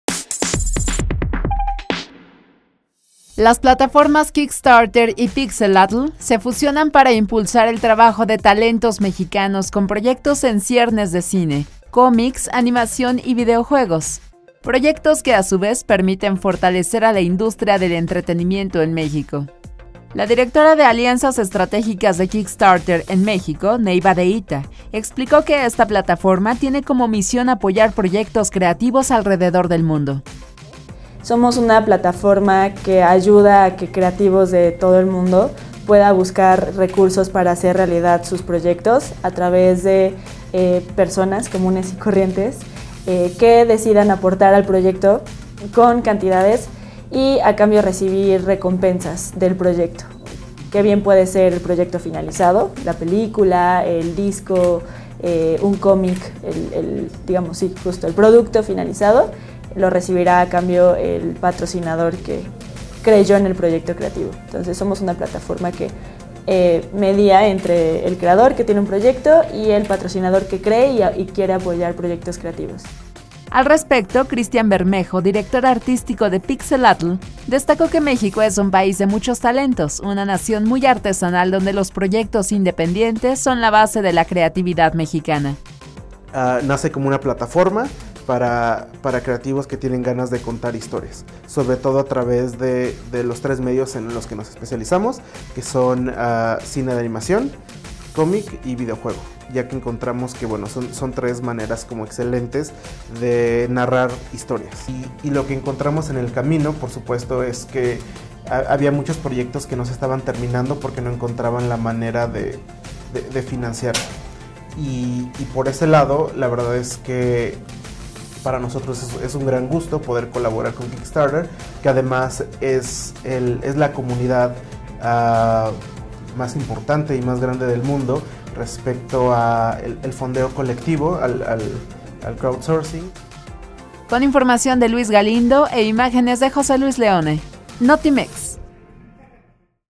Kickstarter y Pixelatl se fusionan para impulsar talento mexicano
Plataformas que financian proyectos de creativos emergentes y consagrados